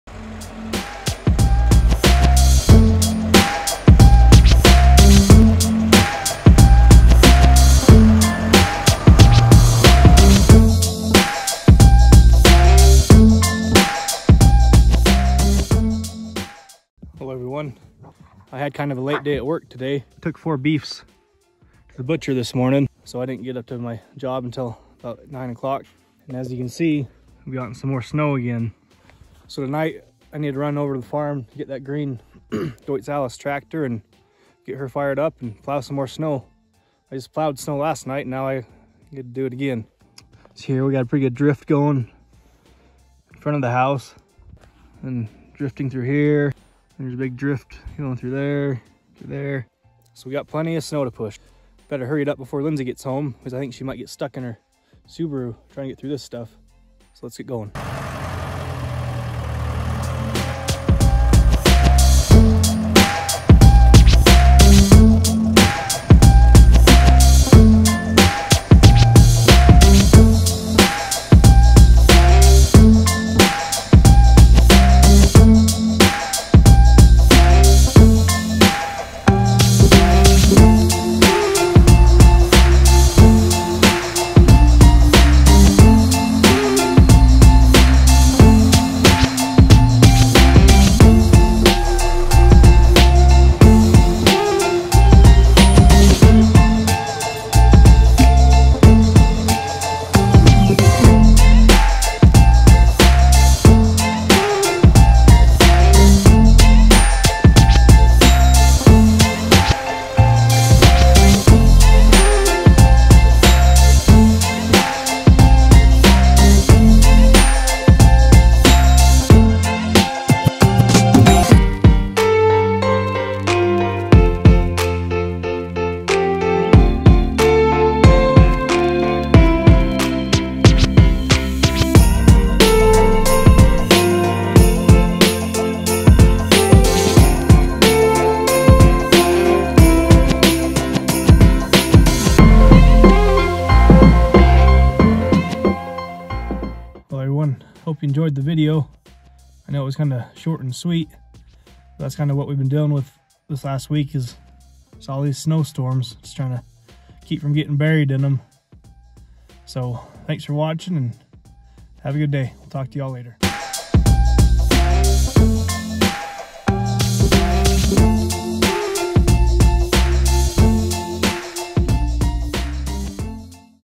Plowing Snow With The Deutz-Allis Tractor
We just got hit by a Wyoming snowstorm in Star Valley, Wyoming. Now it's time to start plowing snow with the tractor. In this quick and fun video I try and show some of our snow removal equipment and the amount of time it takes us to move snow.